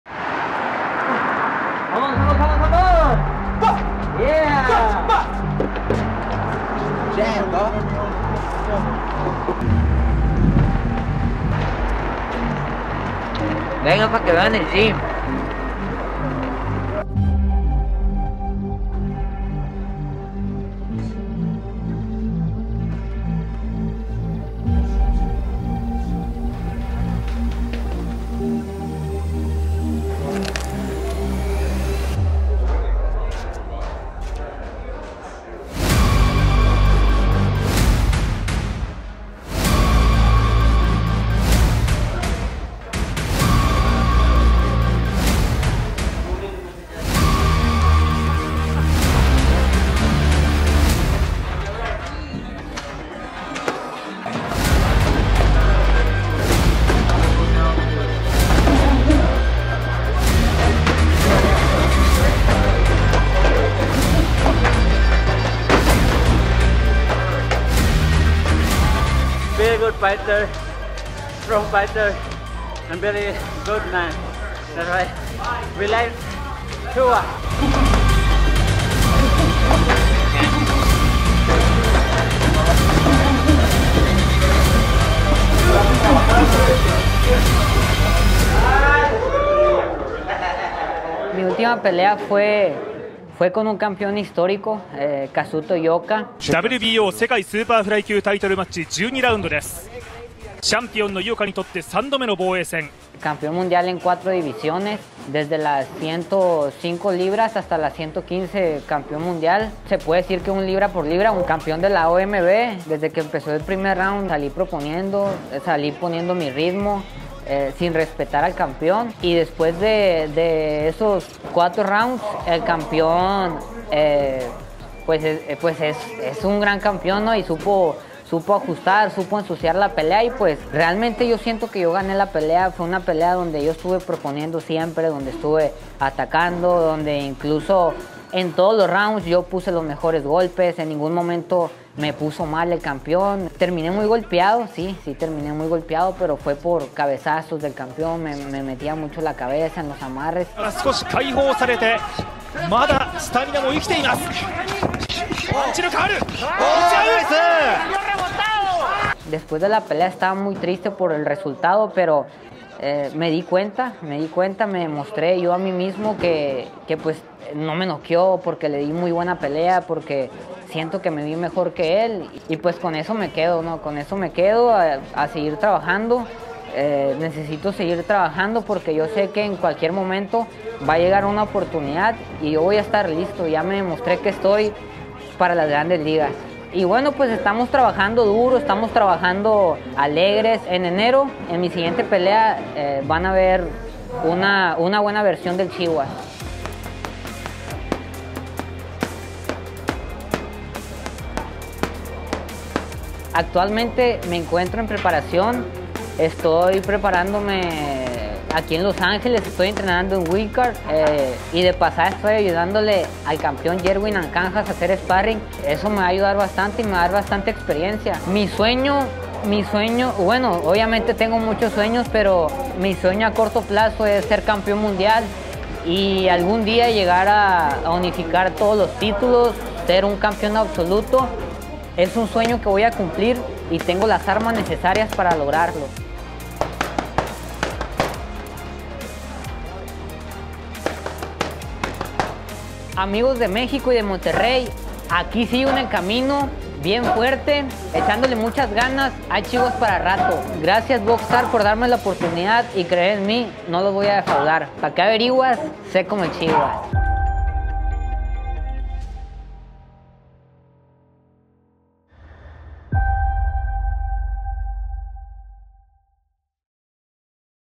Francisco Rodriguez Jr "El Chihuas"
35/5/1
24kos/1Ko

Chihuas enpreparación se encuentra en Los Ángeles trabajando en el Wild Card Boxing con el Campeón Jerwin Ancajas actual Campeón del International Boxing Federation World Super Fly Title, de Filipinas, además nos platica como vivió minuto a minuto el enfrentamiento con el Campeón Kazuto Ioka, en sus palabras: "mi sueño ser campeón mundial, unficar todos los títulos ser Campeón Absoluto, tengo las armas necesarias para lograrlo, estoy listo para las grandes ligas"